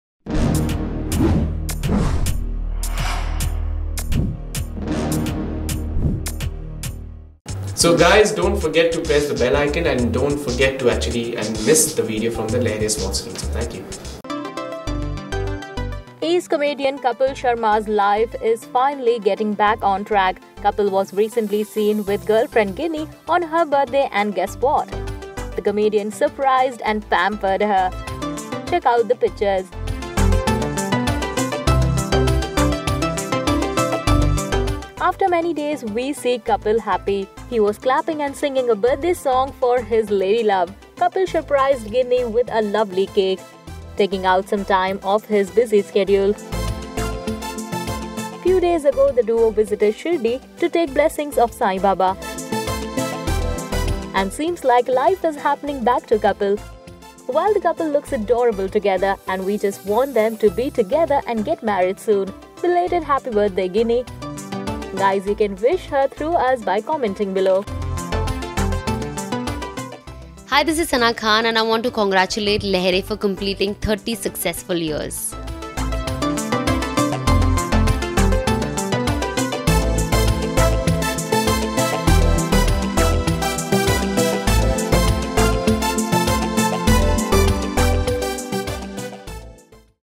Kapil Sharma Surprises Girlfriend Ginni On Her Birthday | Inside Pictures
Ace comedian Kapil Sharma surprised his girlfriend Ginni Chatrath on her birthday.